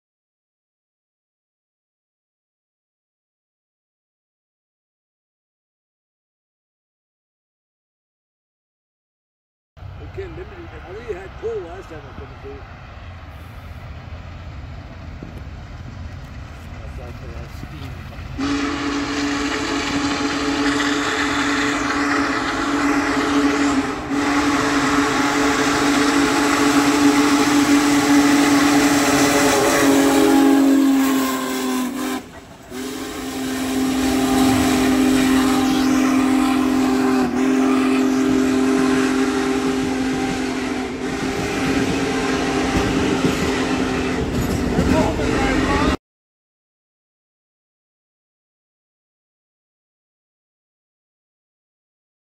bigboyy hinkley
big boy going thru hinkley, MN  at a decent speed july 19th